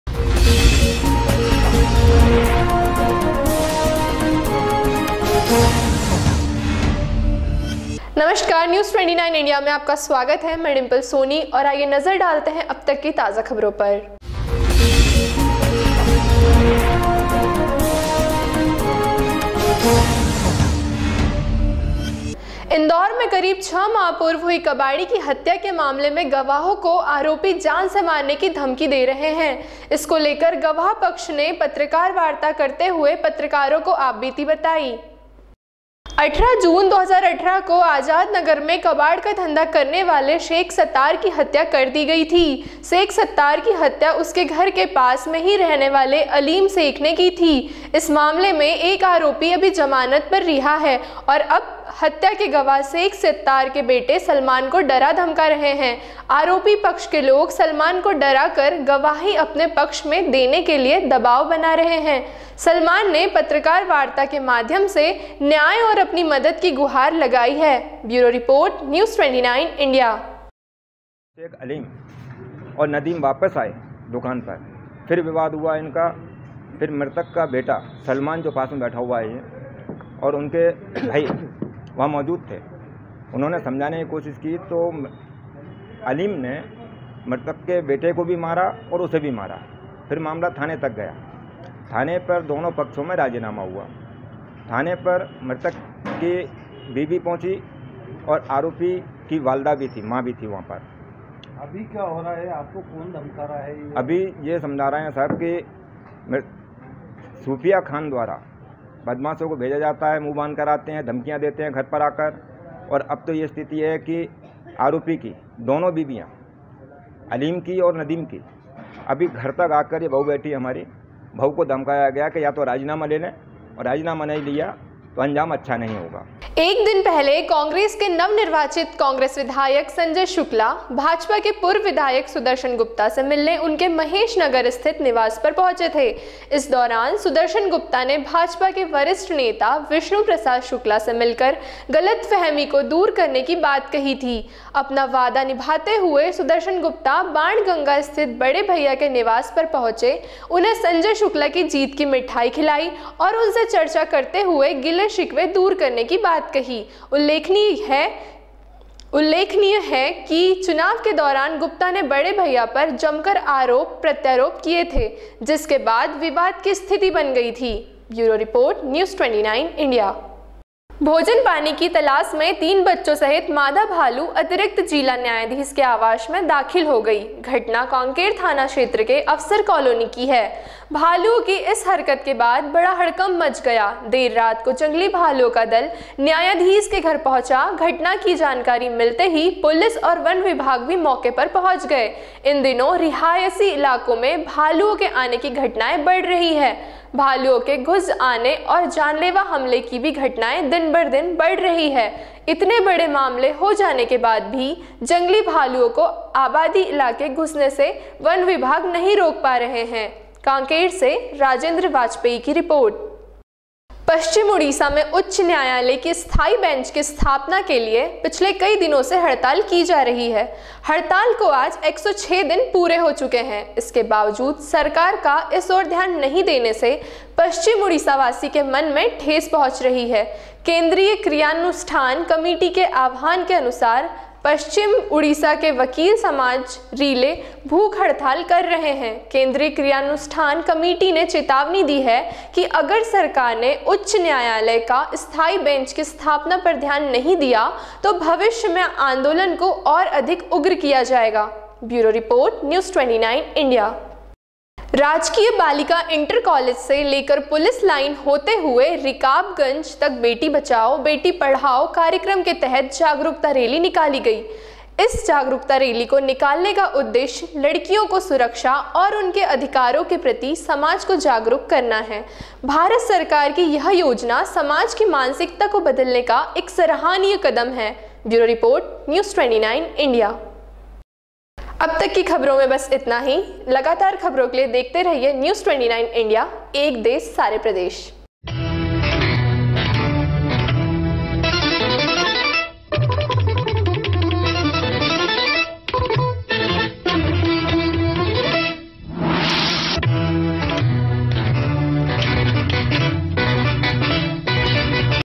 News29India#हत्या के मामले में गवाहों को आरोपी धमका रहे#
इंदौर में करीब छह माह पूर्व हुई कबाड़ी की हत्या के मामले में गवाहों को आरोपी दे रहे जान से मारने की धमकी, गवाह पक्ष ने पत्रकार वार्ता करते हुए पत्रकारों को बताई आपबीती।
 कांग्रेस के नवनिर्वाचित कांग्रेस विधायक संजय शुक्ला और उनके पिता विष्णु प्रसाद षुक्ला से मिलने पहुंचे भाजपा के पूर्व विधायक सुदर्शन गुप्ता, गुप्ता ने विष्णु प्रसाद शुक्ला से मिलकर गलतफहमी दूर करने की बात कही।
 भोजन पानी की तलाश में तीन बच्चों सहित मादा भालू अतिरिक्त जिला न्यायाधीश के आवास में हुआ दाखिल, देर रात को जंगली भालुओं का दल न्यायाधीष के घर पहुंचा।